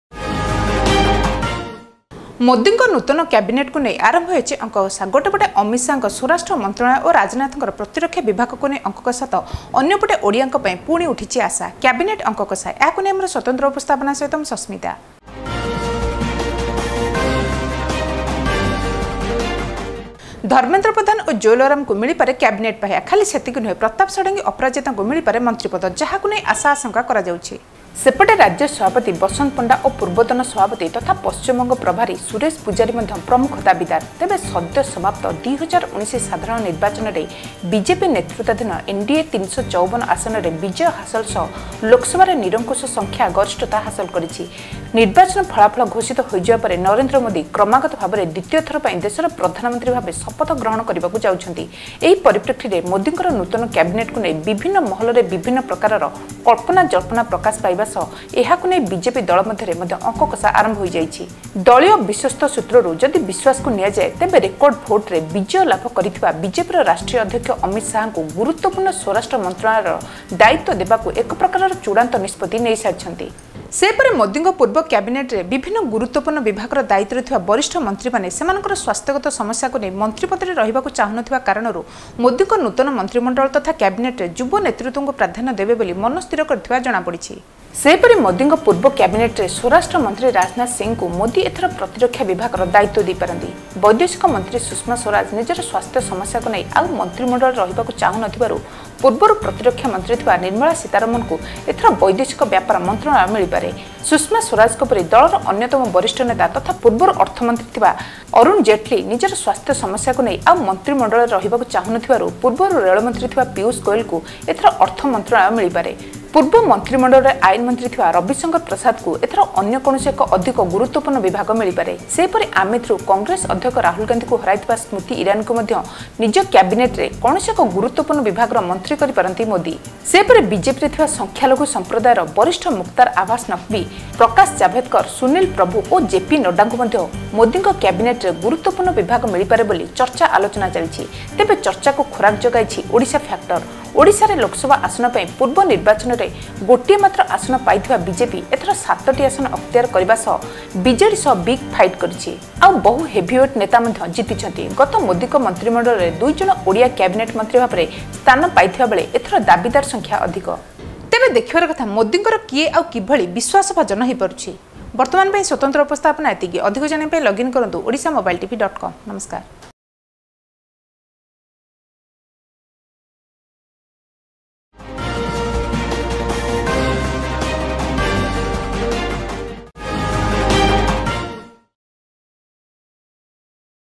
Special News-କ୍ୟାବିନେଟ୍‌ ଅଙ୍କ କଷା ଓଡିଶାରୁ ୩
ମୋଦିଙ୍କ ନୂତନ କ୍ୟାବିନେଟକୁ ନେଇ ଆରମ୍ଭ ହେଲା ଅଙ୍କକଷା । ଗୋଟେ ପଟେ ଅମିତ ଶାହାଙ୍କୁ ସ୍ୱରାଷ୍ଟ୍ର ମନ୍ତ୍ରାଳୟ, ରାଜନାଥଙ୍କୁ ପ୍ରତିରକ୍ଷା ନେଇ ଅଙ୍କ କଷା ତ ଅନ୍ୟ ପଟେ ଓଡିଆ ପାଇଁ ପୁଣି ଉଠିଛି ଆଶା । ଧର୍ମେନ୍ଦ୍ର ଓ ଜୁଏଲଙ୍କ ବ୍ୟତୀତ ଆଉ ଗୋଟିଏ ପଦ ବି ଆଶା